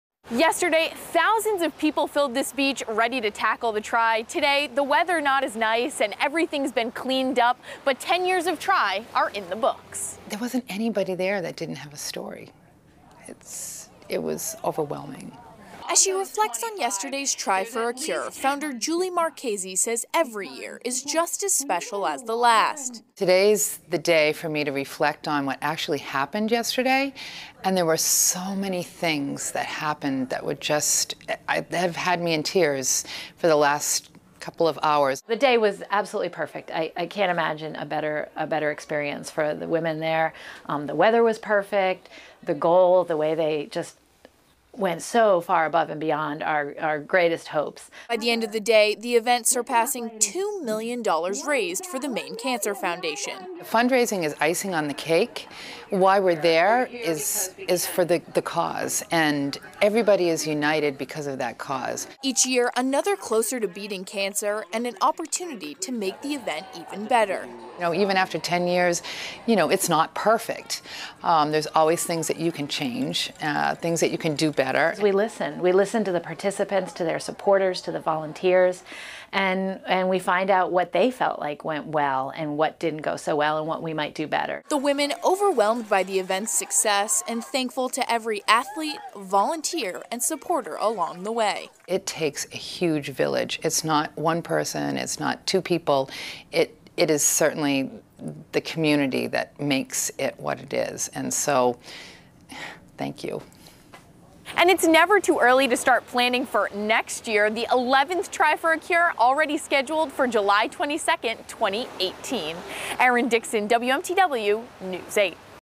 'It was overwhelming' Tri For A Cure smashes fundraising goal
Sunday's Tri For A Cure raised more than $2 million for the Maine Cancer Foundation and the donations keep coming in.